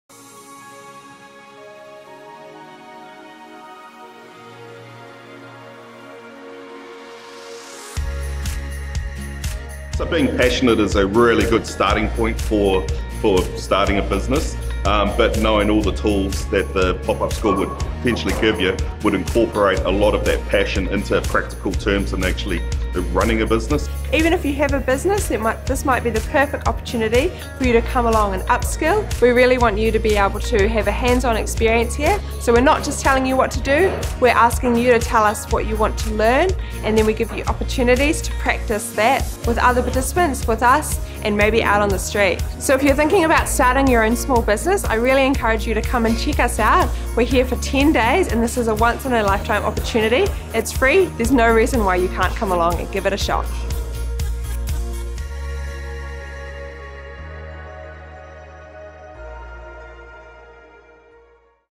Pop Up Business School Upper Hutt Short 2
This brief video talks about the opportunity to participate in the PopUp Business School Aotearoa event to be held in Upper Hutt at the NZ Campus of Innovation and Sport (NZCIS) between the 12-23 March 2018. The PopUp Business School is a FREE 10-day event with 20 workshops where participants get real with bringing their business idea to life.